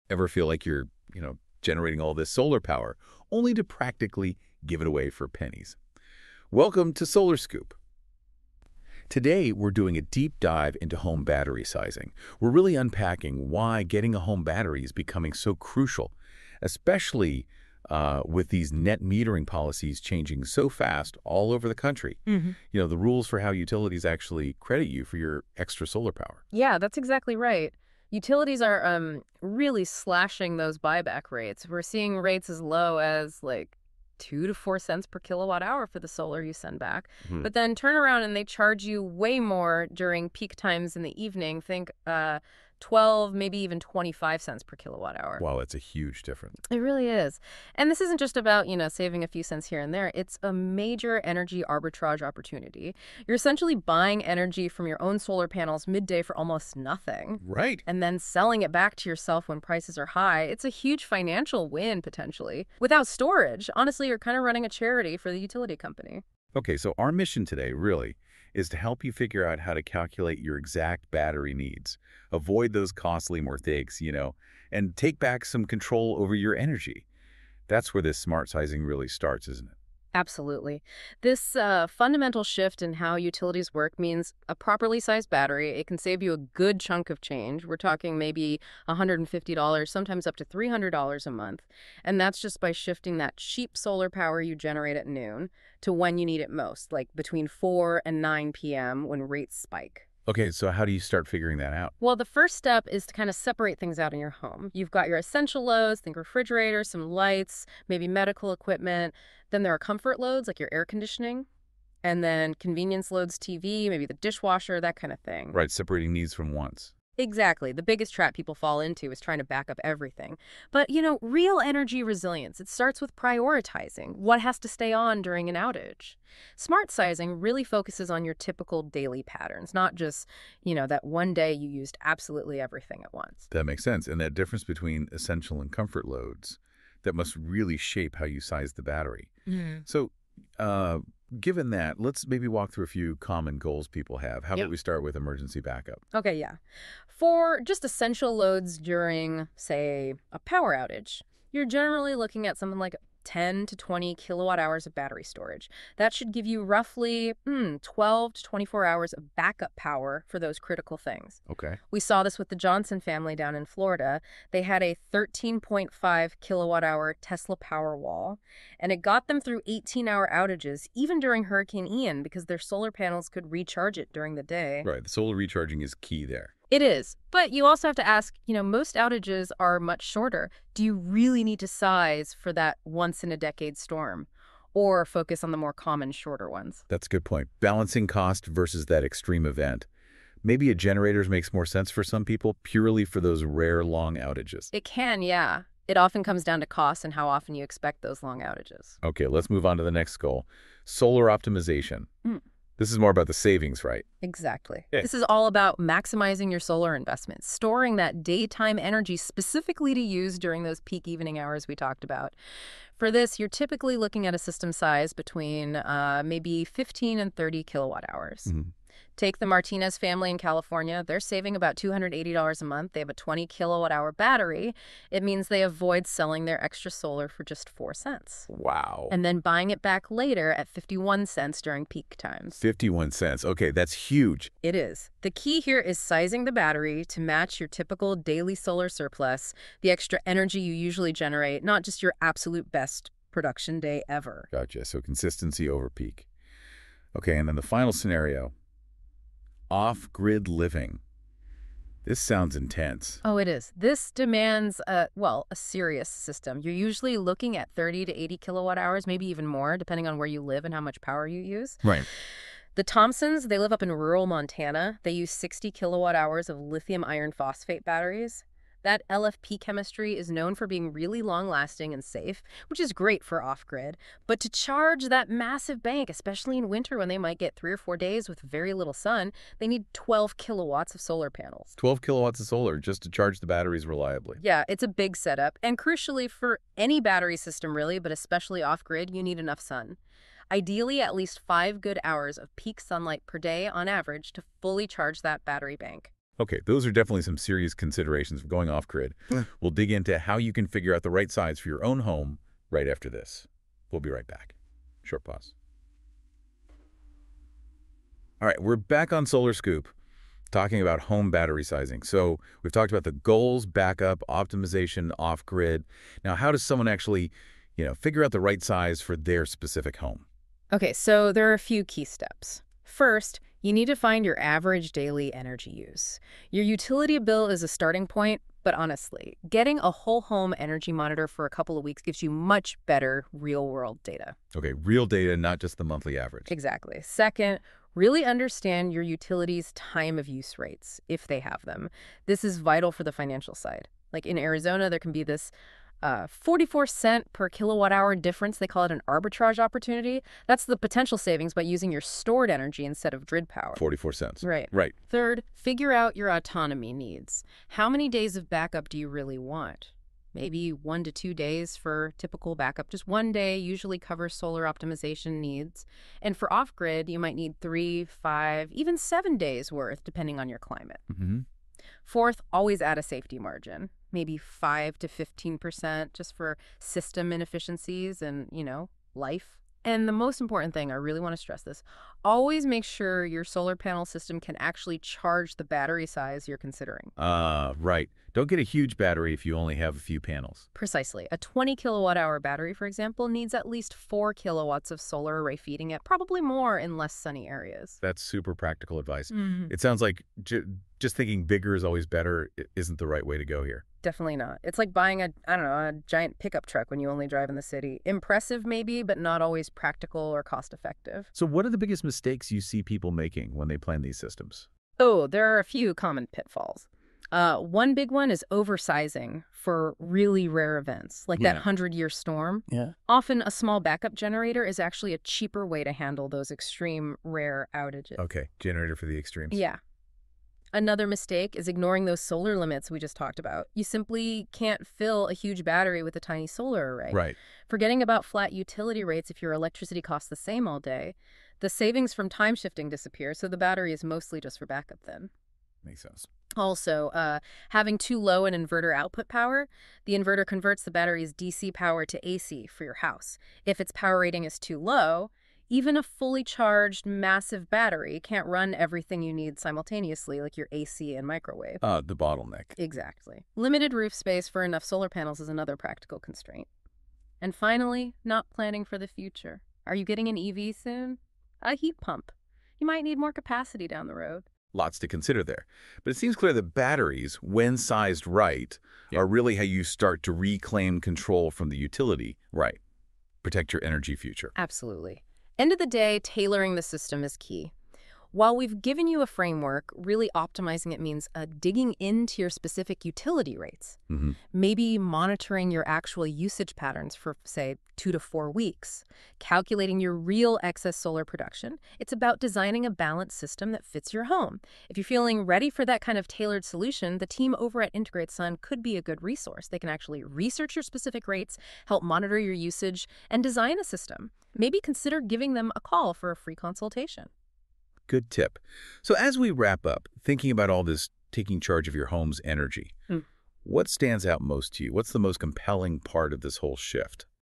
Stop Giving Your Solar Power Away for Pennies
Are you generating solar power only to practically give it away for pennies? With net metering policies changing rapidly across the country, utilities are slashing buyback rates to as low as 2-4 cents per kWh while charging you 12-25 cents during peak hours. This creates a massive energy arbitrage opportunity - if you know how to size your battery system correctly.

In this essential episode, we break down the science and strategy behind home battery sizing. Whether you're looking for emergency backup, solar optimization, or complete energy independence, we'll help you calculate your exact battery needs and avoid costly mistakes that could leave you running a charity for your utility company.

From essential vs. comfort loads to real-world case studies, we're giving you the framework to take back control of your energy future. Because without proper battery storage, you're essentially buying energy from your own solar panels for almost nothing and selling it back to yourself when prices are high.

Key Timestamps

0:00 - The net metering problem: Giving away solar power for pennies
 0:22 - Utility rate arbitrage: 2-4 cents out vs. 12-25 cents in
 1:17 - Why proper sizing can save $150-300 monthly
 1:42 - Essential vs. comfort vs. convenience loads framework
 2:20 - Emergency backup sizing: 10-20 kWh for 12-24 hours
 3:22 - Solar optimization: 15-30 kWh for maximum savings
 4:09 - Off-grid living: 30-80+ kWh for complete energy independence
 5:22 - How to calculate your specific battery needs
 5:45 - The Arizona example: 44 cents per kWh arbitrage opportunity
 6:55 - Common sizing mistakes that cost money
 7:10 - Generator vs. battery for extreme events
 8:03 - Taking back control from utilities

Key Takeaways

🔋 Emergency Backup: 10-20 kWh covers essential loads for 12-24 hours
 ⚡ Solar Optimization: 15-30 kWh maximizes savings from time-of-use rates
 🏠 Off-Grid Living: 30-80+ kWh for complete energy independence
 💰 Financial Impact: Properly sized systems can save $150-300+ monthly
 🎯 Sizing Strategy: Match battery to daily solar surplus, not peak production
 ⚡ Power Matching: Ensure adequate solar array (minimum 4 kW for 20 kWh battery)

Essential Sizing Steps

  1. Calculate average daily energy use (use home energy monitor, not just bills)
  2. Understand your utility's time-of-use rates and arbitrage opportunities
  3. Determine your autonomy needs (1-2 days backup, 3-7 days off-grid)
  4. Add 5-15% safety margin for system inefficiencies
  5. Ensure adequate solar array to charge your battery system

Ready for a customized battery sizing solution? Contact IntegrateSun for a free consultation to analyze your specific rates and usage patterns.